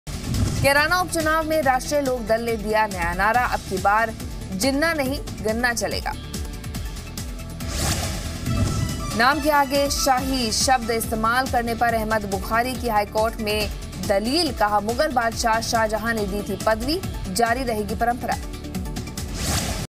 Shahi Imam title given by Shahjahan: Syed Ahmed Bukhari
Shahi Imam title given by Shahjahan: Syed Ahmed Bukhari . Watch the video to know other top news of the day.